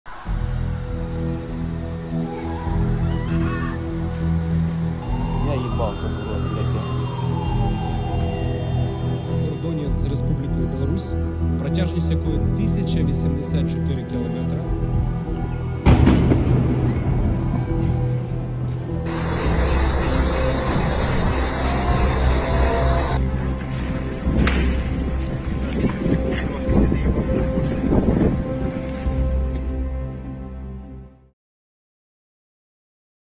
Ukrayna'da helikopter kazası: İçişleri Bakanı öldü
Ukrayna'da, Kiev yakınlarındaki Brovary'de bir helikopter düştü. Kazada, Ukrayna İçişleri Bakanı hayatını kaybetti.